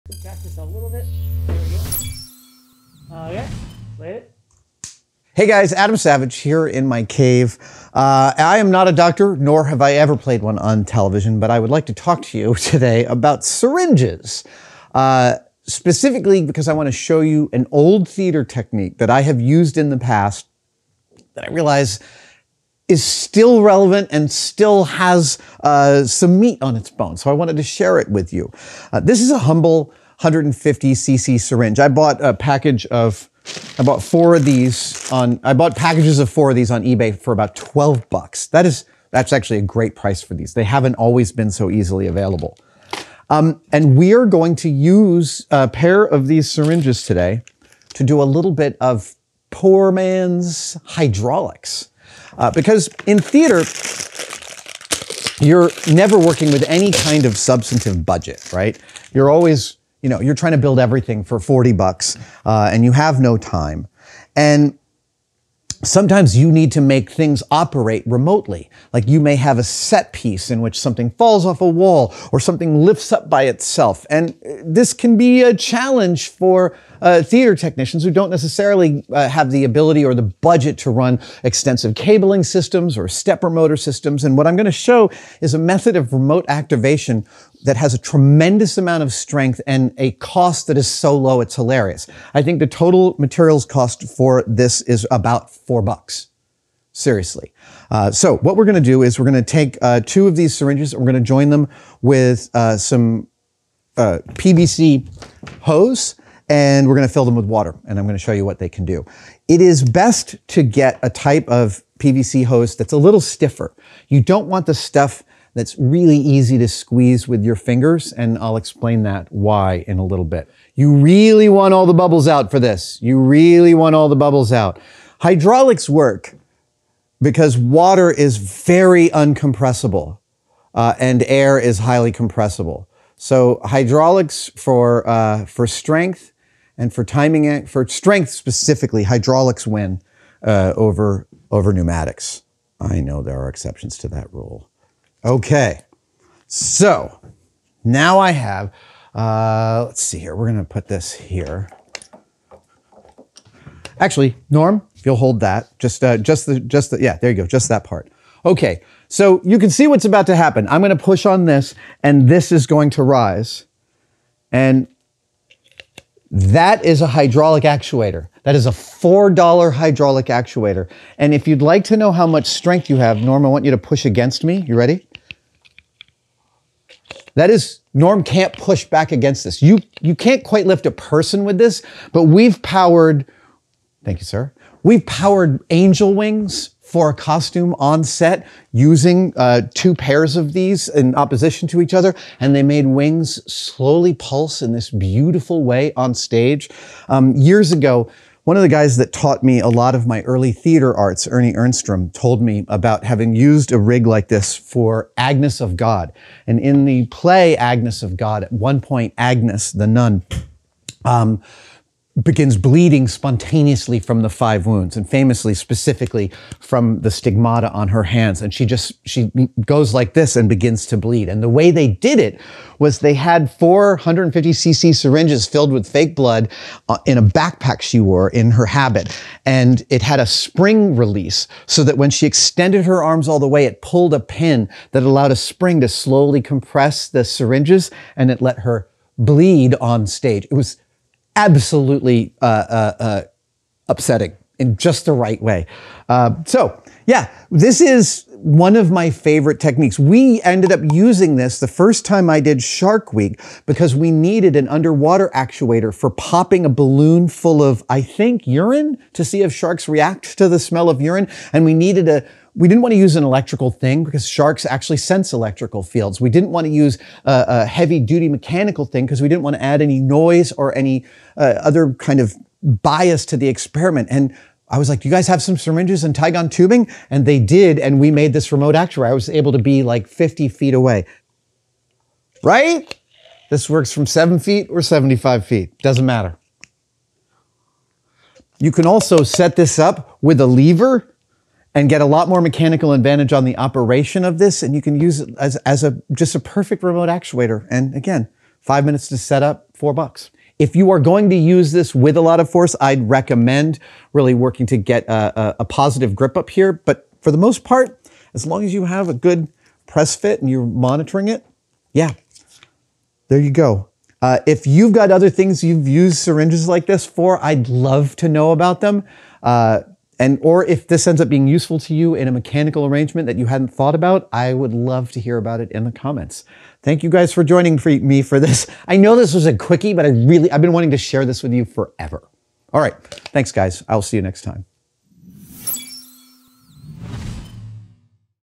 Adam Savage Demos Hydraulics with Cheap Plastic Syringes
Adam shows us how to use cheap plastic syringes to create a simple hydraulics system for remote activation and actuation. It's an effective technique used extensive in his early theater days, and can create compelling practical stage effects with very little budget!